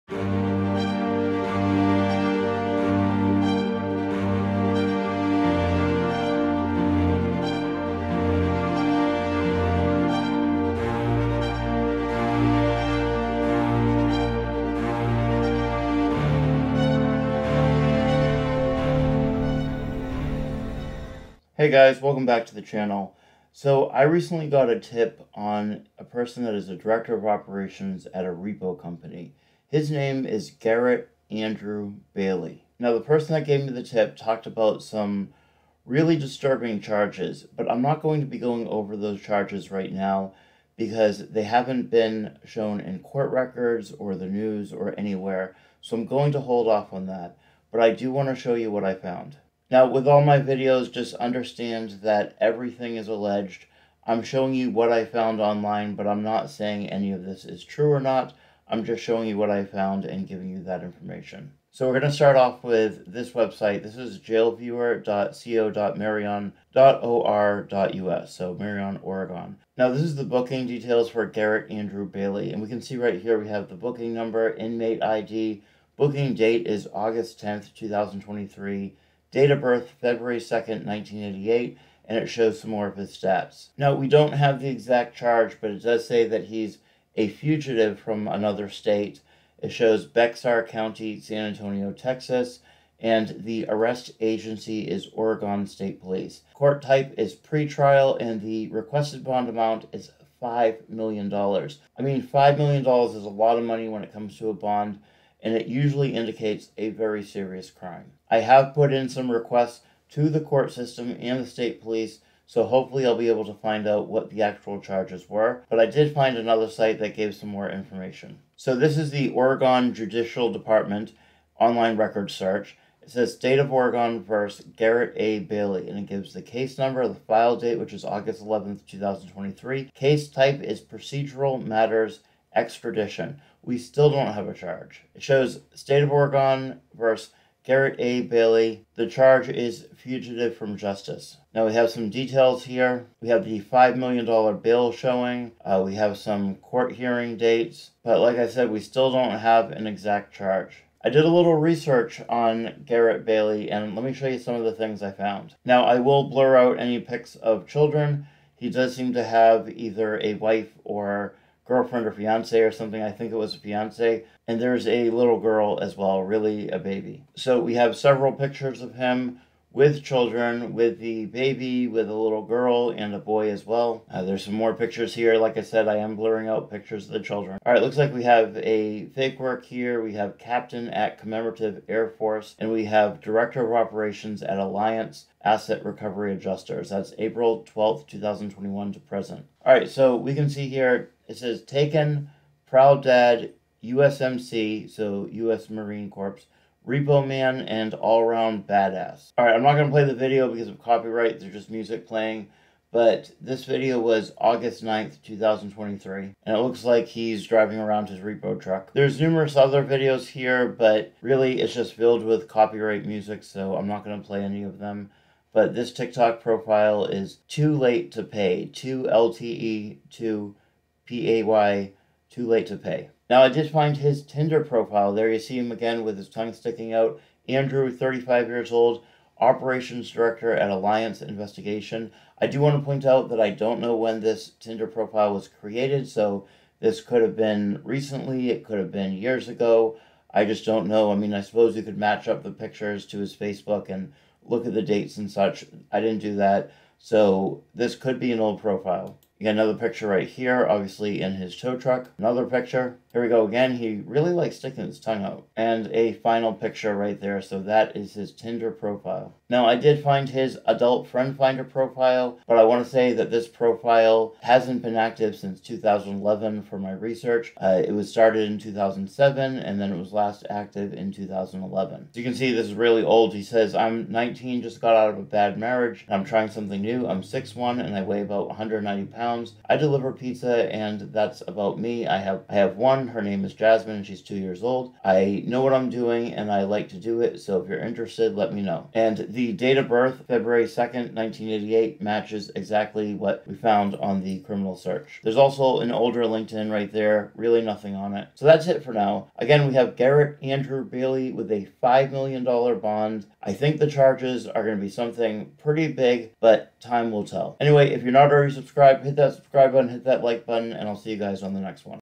Fugitive Repo Agent Gets Arrested!!
Garrett Bailey, a long time repo agent just got arrested and hit with a 5 million dollar bond!

$15 VIP frequent behind the scenes content and exclusive videos